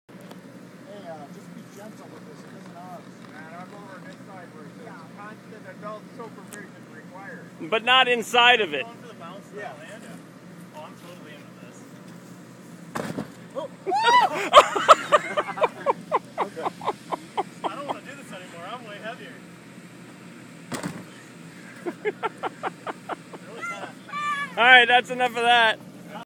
Inflatable Jump Fall
The dirt and grass are not as forgiving as the inflated slide. :)

I should have gone second...